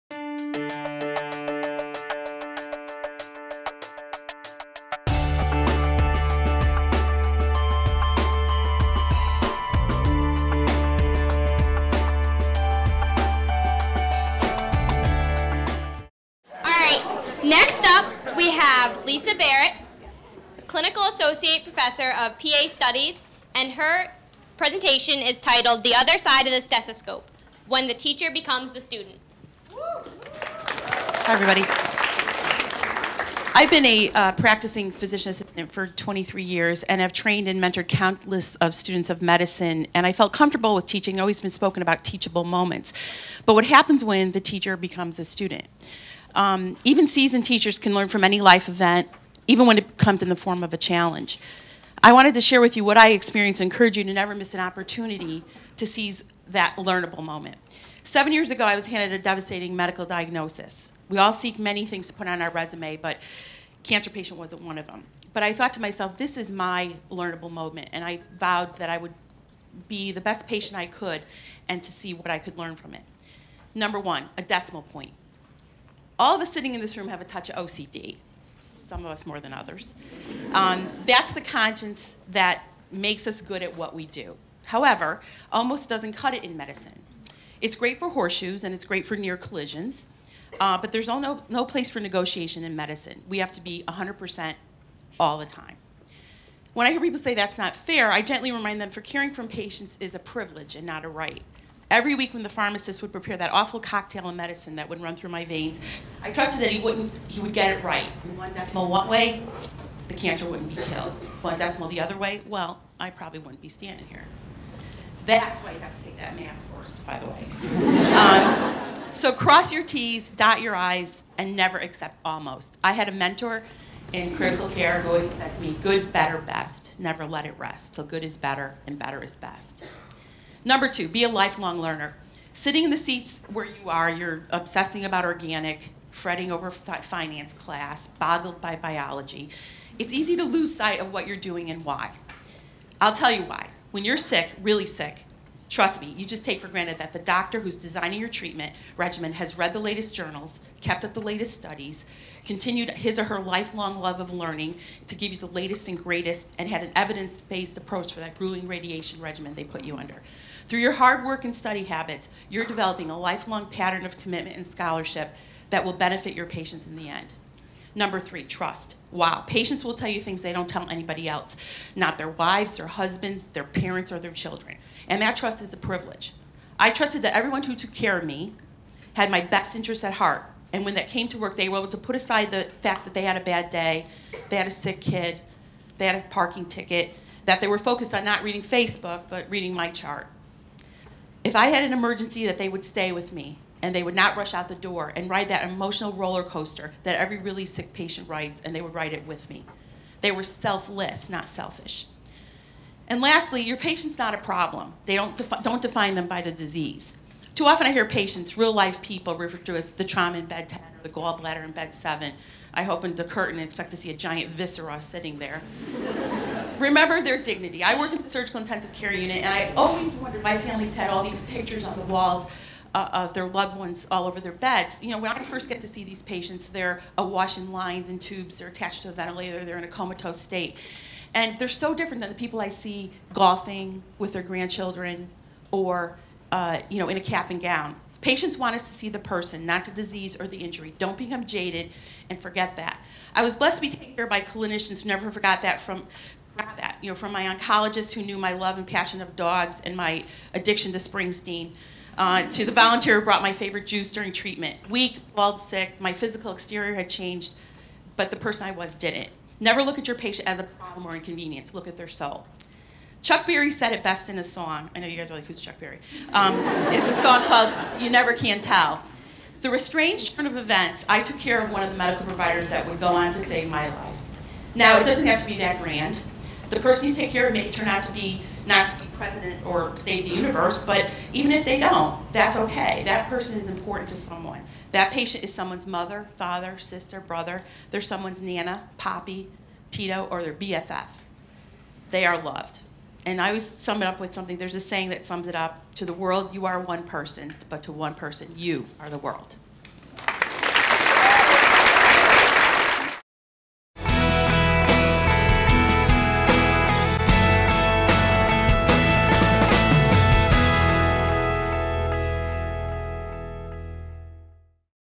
60 Second Lecture Series - "The Other Side of the Stethoscope" - Lisa Barratt
On March 1, 2014,  Clinical Associate Professor of PA Studies Lisa Barratt presented, "The Other Side of the Stethoscope: When the Teacher Becomes the Student." This lecture was part of the "60 Second Lecture Series" which was hosted by Quinnipiac University's Honors Program."